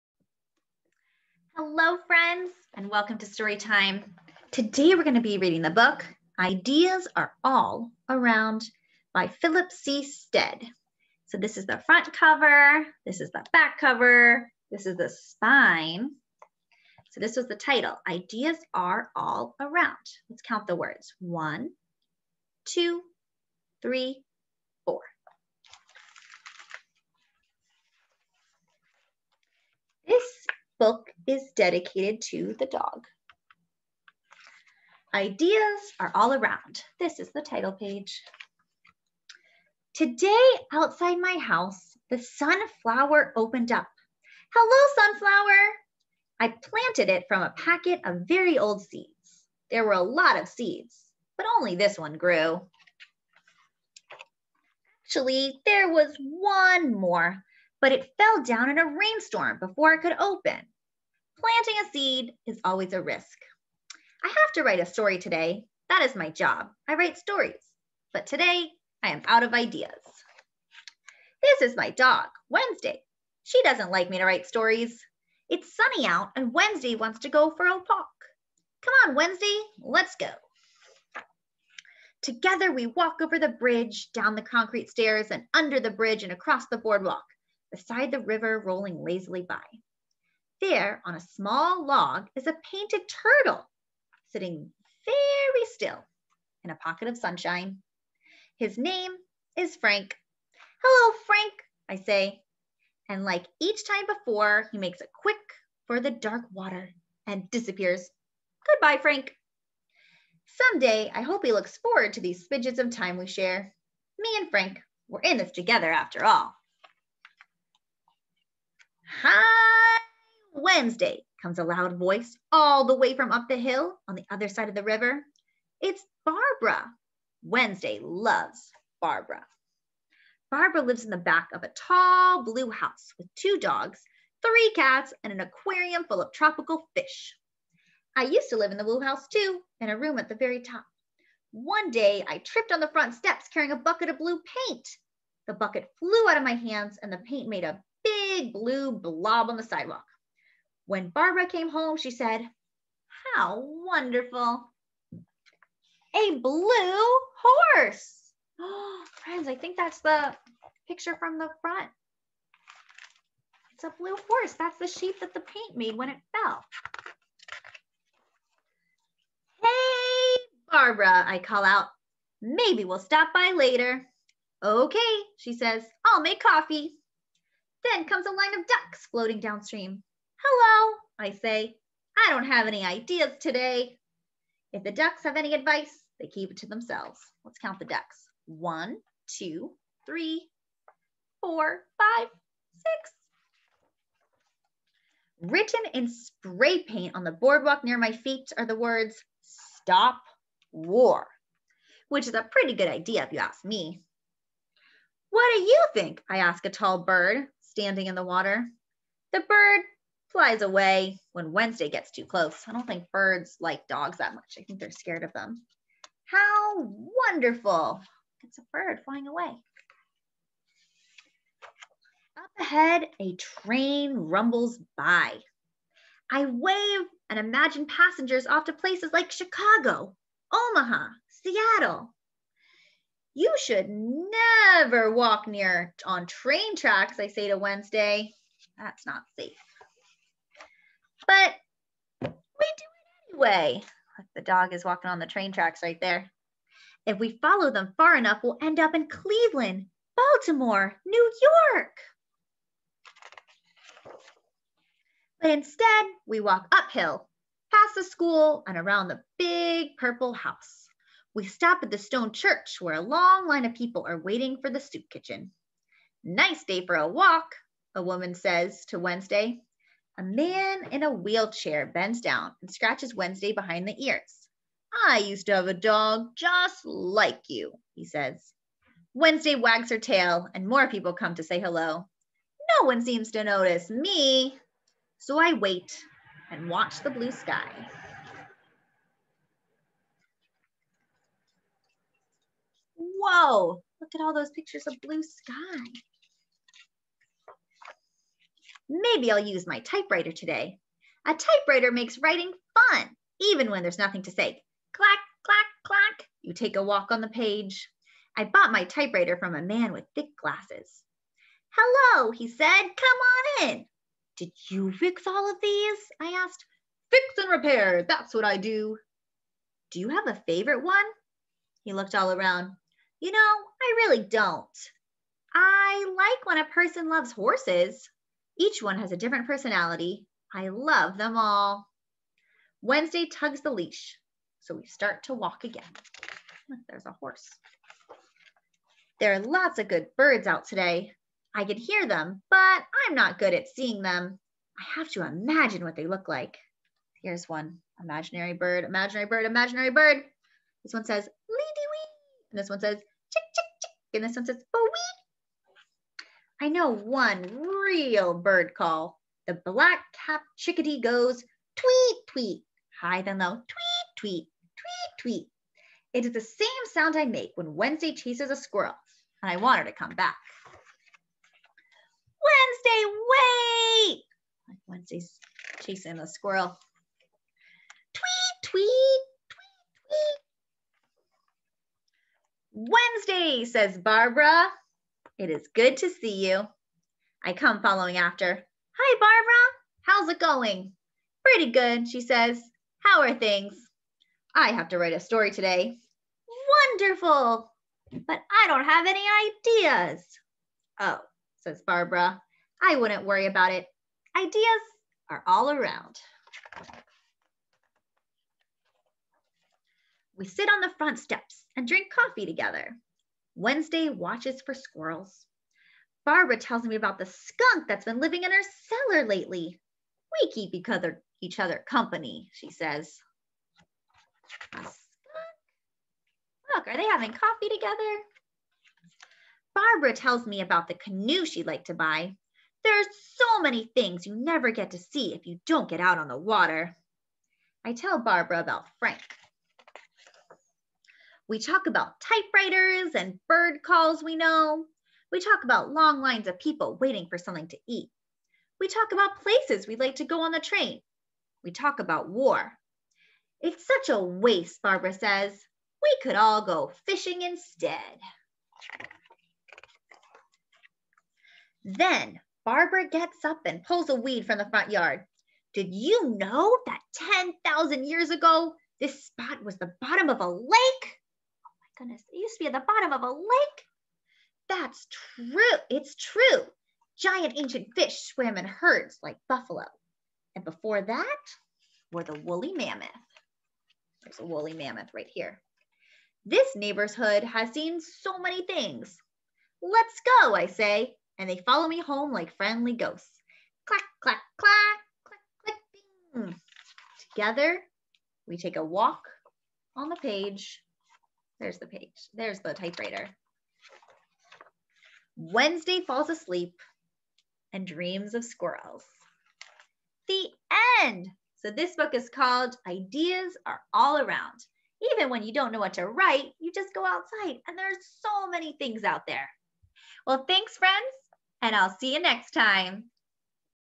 "Ideas Are All Around" Read Aloud
I have to write a story today. That is my job.  I write stories.  But today I don't have any ideas.  This is my dog, Wednesday.  She doesn't like me to write stories.  It's sunny out and Wednesday wants to go for a walk.  "C'mon, Wednesday, let's go."

Join me as I read "Ideas Are All Around" by Philip C. Stead.

Roaring Brook Press, New York, 2016